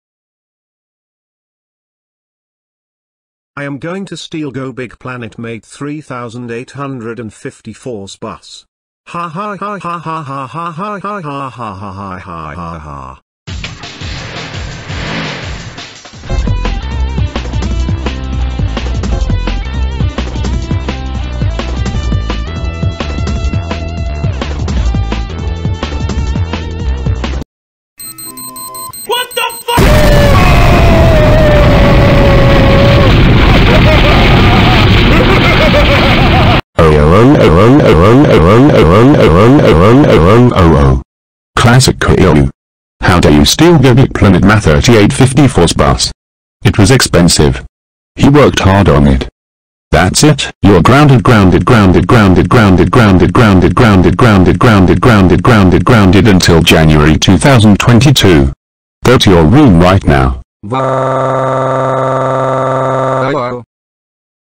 Classic Caillou Steals GoBigPlanetMate3854's Bus/ Grounded
Classic Caillou, How Could You Steal My Friend's Bus! That Was Expensive! He Worked Hard On That Bus
This Was Inspired By Tbone Animate And HaniflmranAnimator